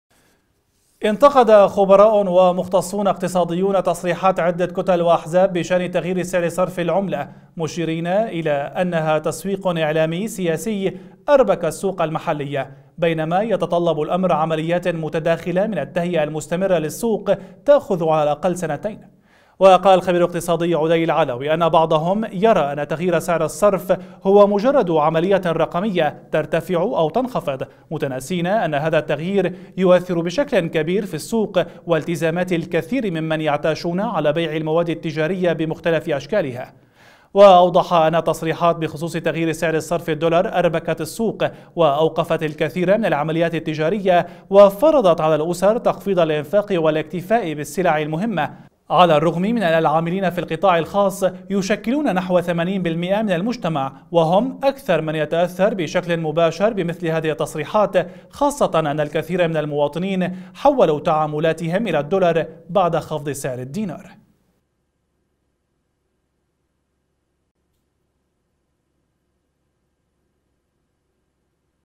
اقتصاديون: تغيير سعر الصرف ليس خيارا سياسيا للتسويق الاعلامي
انتقد خبراء ومختصون اقتصاديون تصريحات عدة كتل وأحزاب بشأن تغيير سعر صرف العملة، مشيرين إلى أنها تسويق إعلامي سياسي أربك السوق المحلية، بينما يتطلب الأمر عمليات متداخلة من التهيئة المستمرة للسوق تأخذ على الأقل سنتين؛ وقال الخبير الاقتصادي عدي العلوي إن بعضهم يرى أن تغيير سعر الصرف هو مجرد عملية رقمية ترتفع أو تنخفض، متناسين أن هذا التغيير يؤثر بشكل كبير في السوق والتزامات الكثير ممن يعتاشون على بيع المواد التجارية بمختلف أشكالها؛ وأوضح أن التصريحات بخصوص تغيير سعر صرف الدولار أربكت السوق وأوقفت الكثير من العمليات التجارية، وفرضت على الأسر تخفيض الإنفاق والاكتفاء بالسلع المهمة، على الرغم من أن العاملين في القطاع الخاص يشكلون نحو ثمانين بالمئة من المجتمع وهم أكثر من يتأثر بشكل مباشر بمثل هذه التصريحات، خاصة أن الكثير من المواطنين حولوا تعاملاتهم إلى الدولار بعد خفض سعر الدينار.